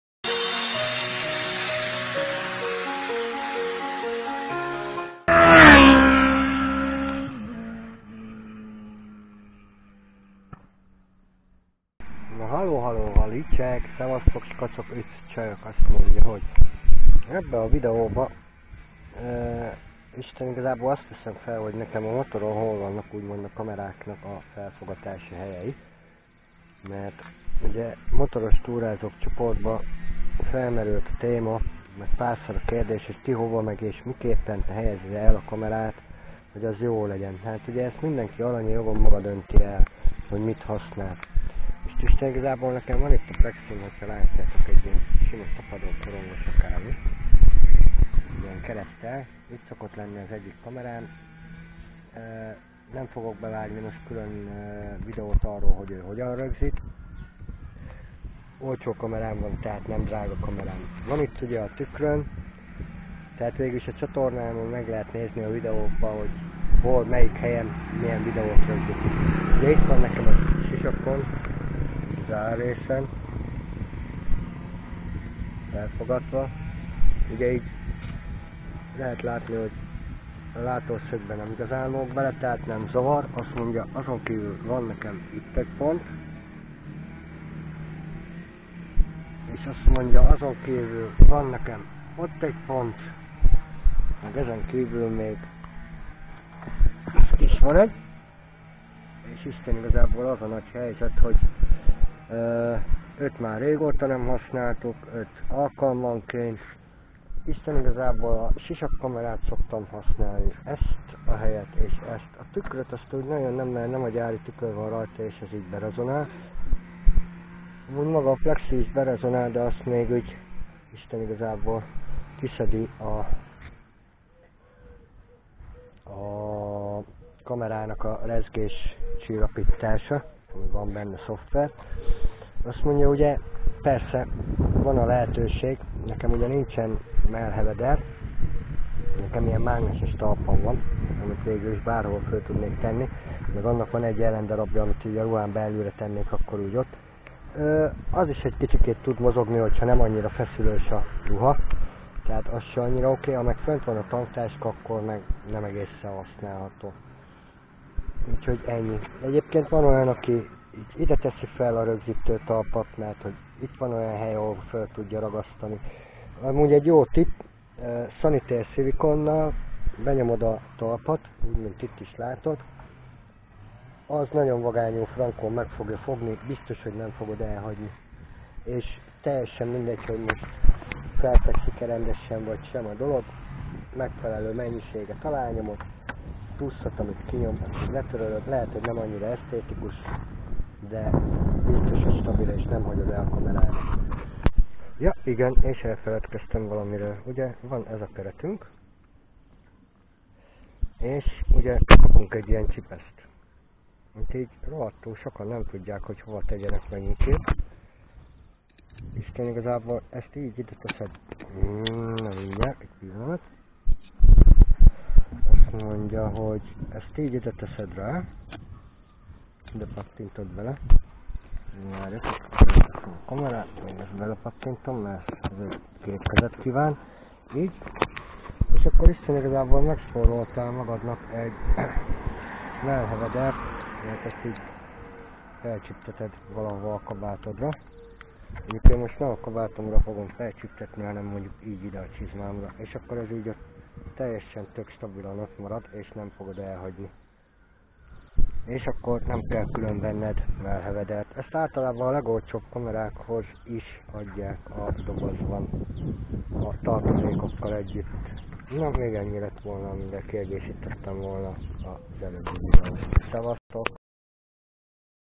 Kamera pontok a Motoromon
Sziasztok !

Lehet  nem  éppen  a  legjobb  videó ,  meg  nem  is  olyan  tudományós,  de  esetleg  ötletekett  addhat  azoknak ,  akik   még  csak  most  gondolkoznak  a  videózáson. A videóban  elhangzik ,  hogy  egy  Face  csoportban elhangzott  kérdéssre   probáltam   választ  és  ötleteket addni .

Megosztást  felíratkozást  köszi ,  és  Face , insta ... stb.